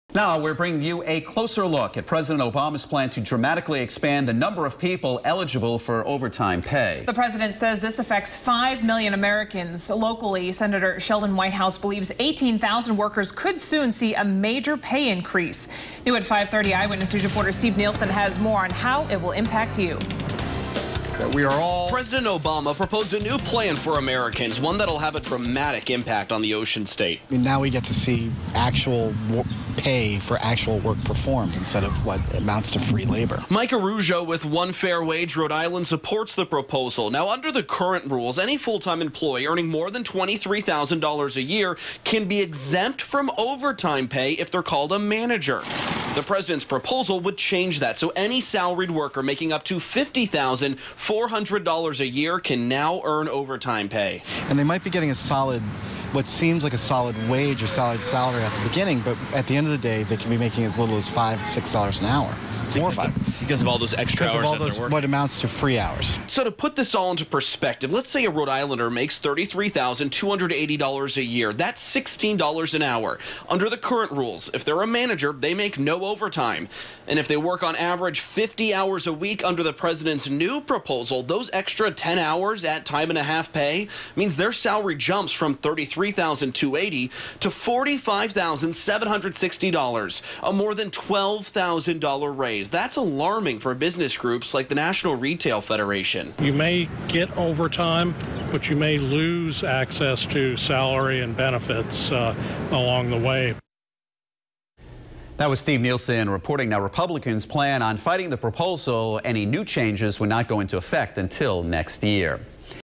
Leaders React To Obama Overtime Proposal
U.S. Senator Sheldon Whitehouse released a statement Tuesday, applauding the new overtime rule.